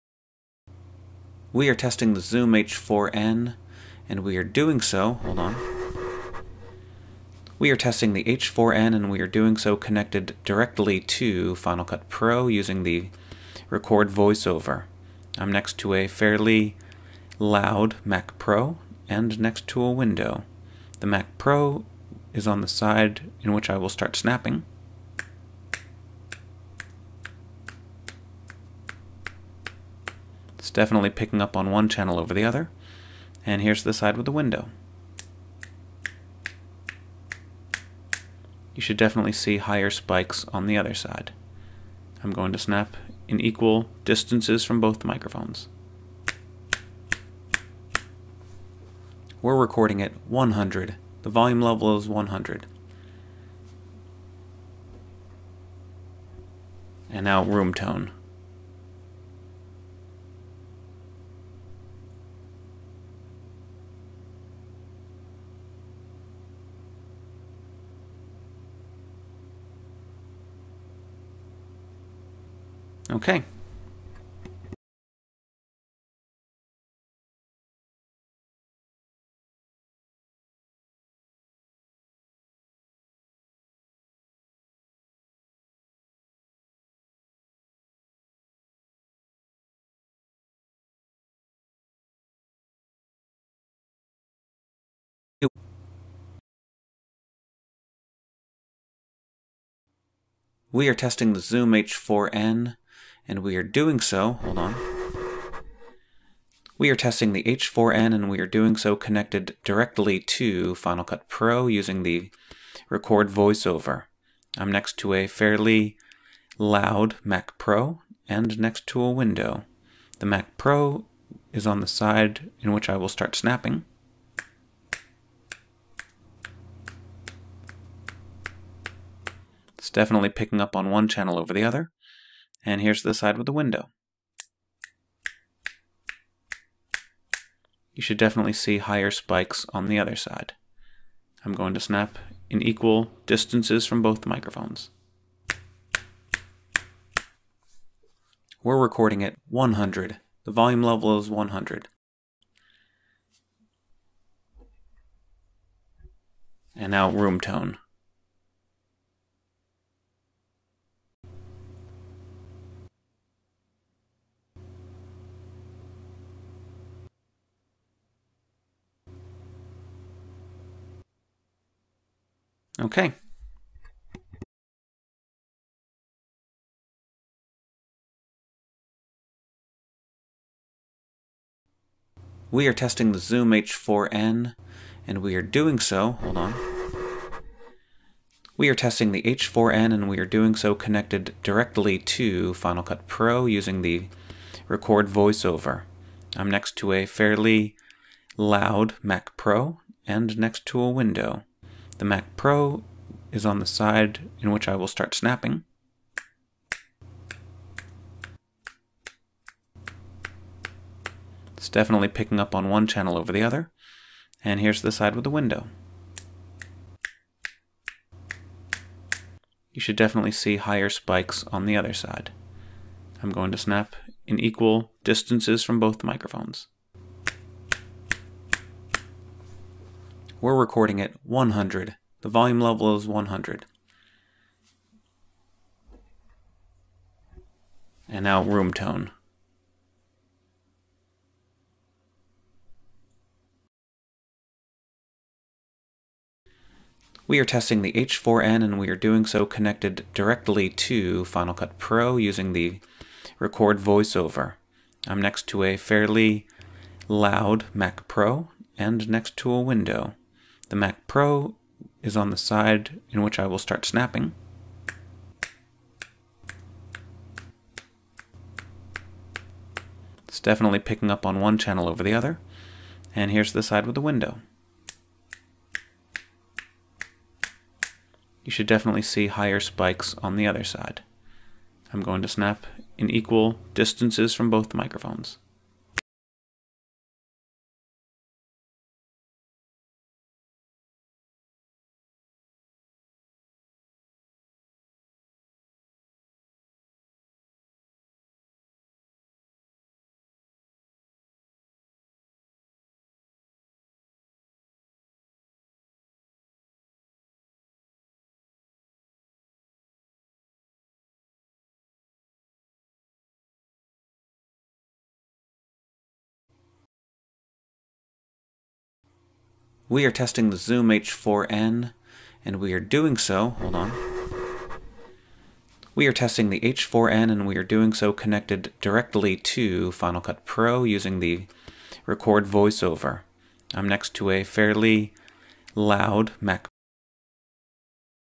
Zoom H4n And Izotope RX 5 Noise Reduction Test
In this video, you can hear the difference made when I apply noise reduction by Izotope's RX 5 software onto a clip of audio recorded by an H4n in a room with a noisy computer (and a fairly quiet street outside).

The Zoom H4n was using just internal microphones and was recording at a level 100 - which everyone says "DON'T!" to because the preamp in the H4n is much too noisy. I wanted to see how RX 5 would be able to handle such noise - not just the preamp's noise but room tone AND a humming computer.

I must say, I'm very impressed!